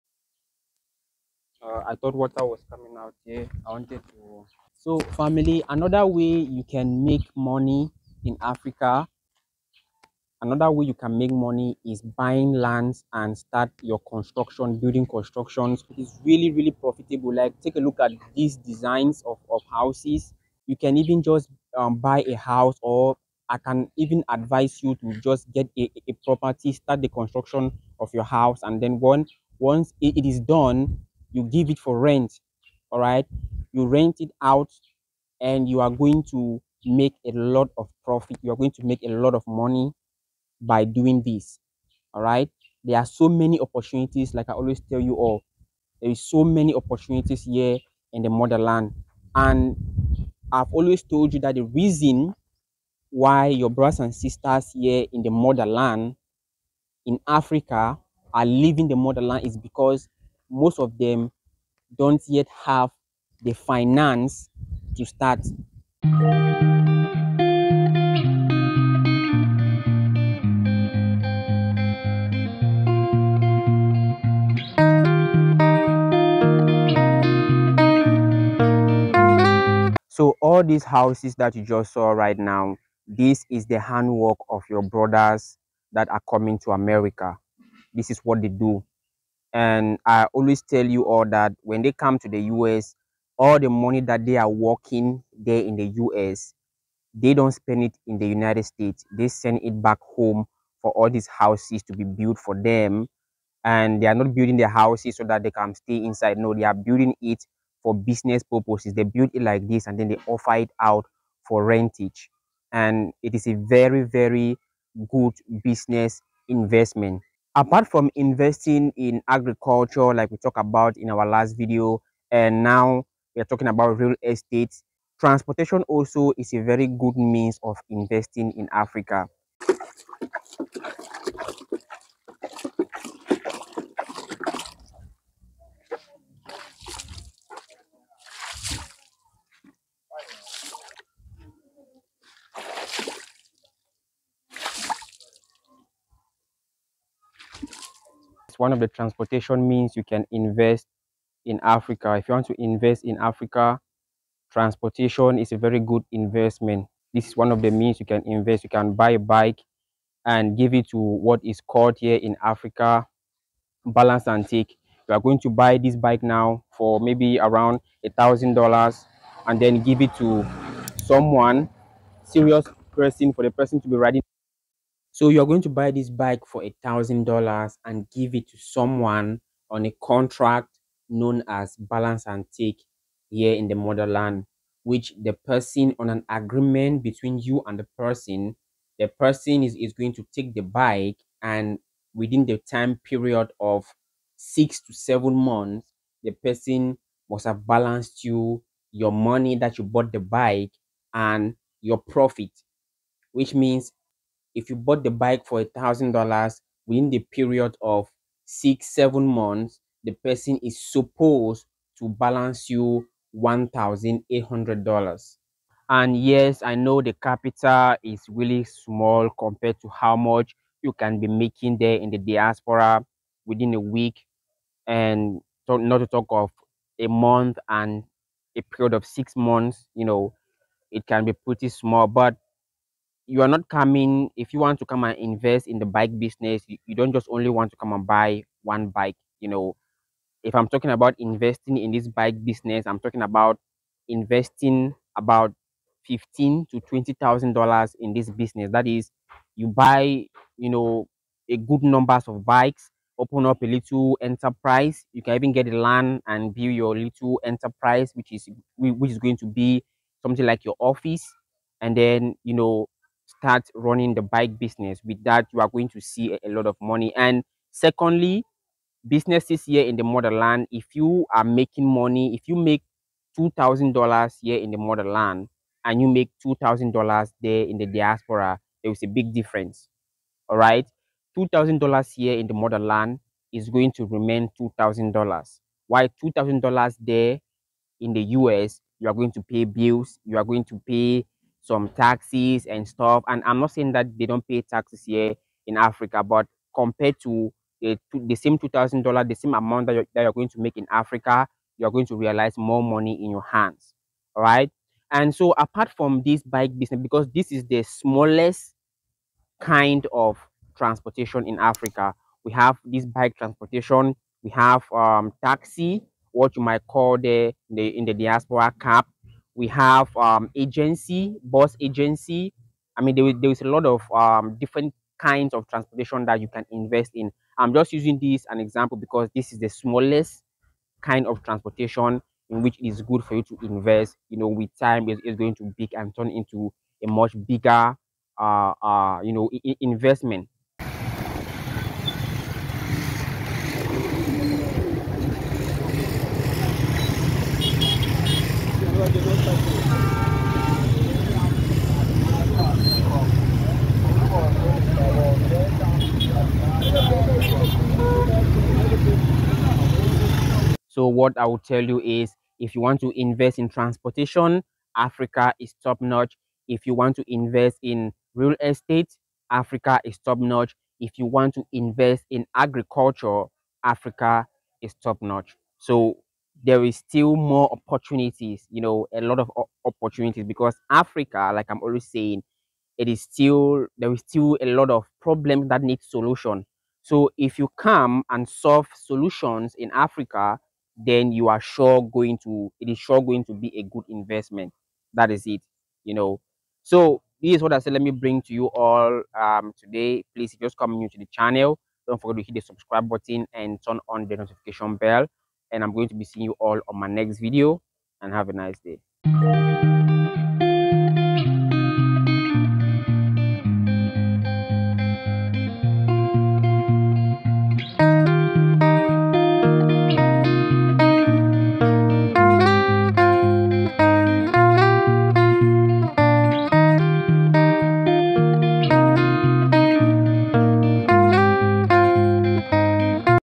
Investing In Africa For African American And The African Diaspora
This Video Is For African Americans And To The African Diaspora At Large. How African Americans And The African Diaspora At Large Can Invest In Africa Without Any Worries.

   African Still Has Alot Of Unsolved Problems Which Solving Them Through Investing Is A Very Good Idea.